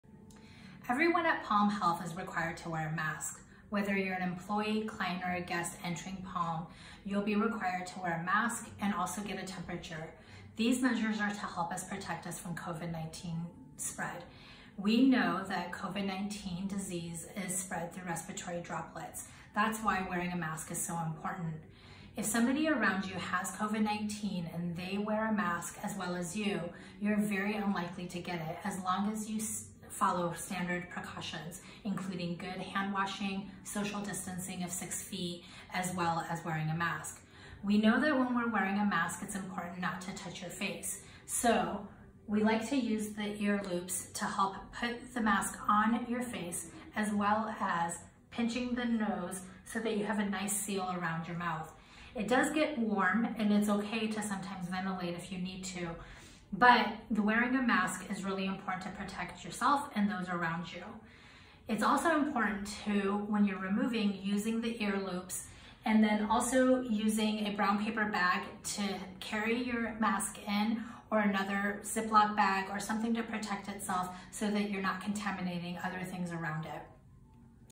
Face Mask Best Practices with Dr. Sita Kedia
Chief Medical Officer Dr. Sita Kedia demonstrates best practices for wearing a face mask and provides helpful information about protecting yourself and others from COVID-19.